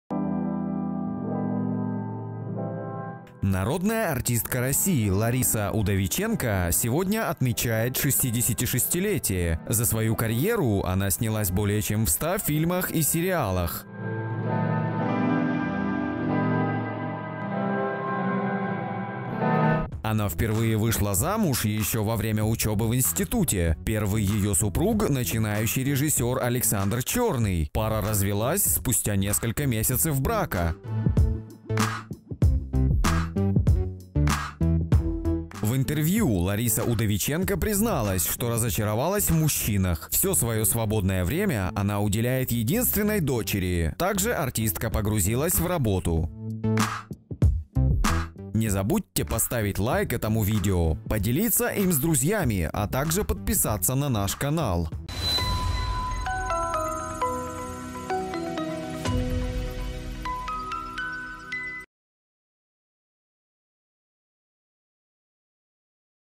Актриса Лариса Удовиченко призналась, что разочаровалась в мужчинах / RuNews24
Российская актриса Лариса Удовиченко разочаровалась в мужчинах после трех неудачных браков.